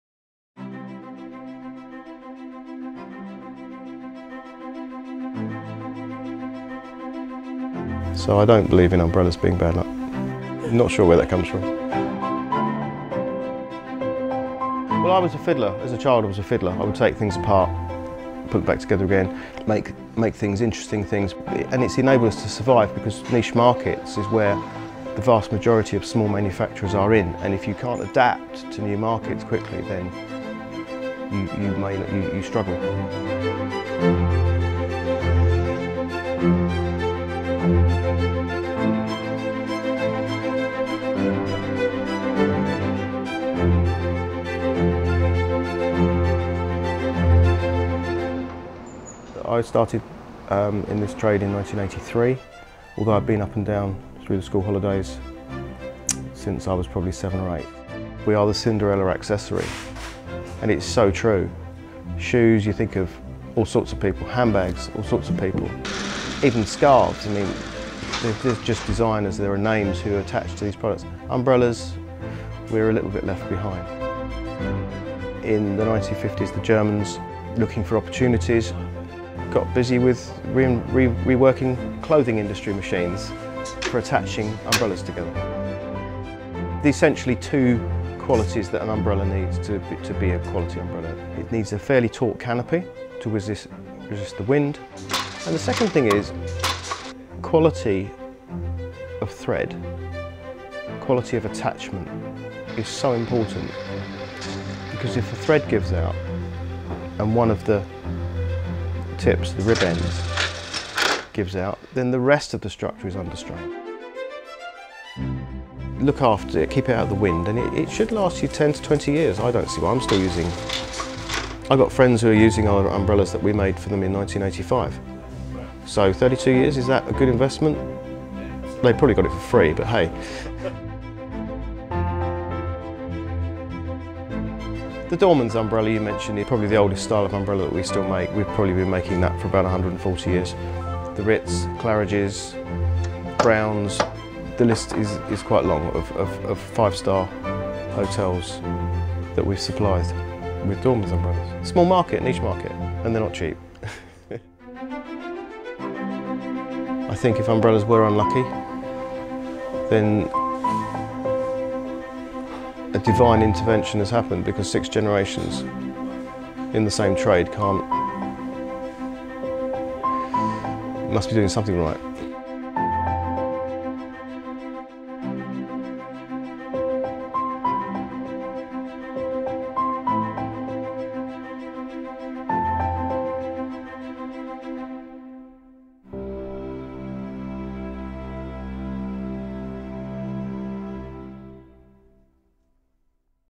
Darby Made | Ince Umbrellas
Trailer for a "Meet the Maker" video series.
BlackMagic
DaVinci Resolve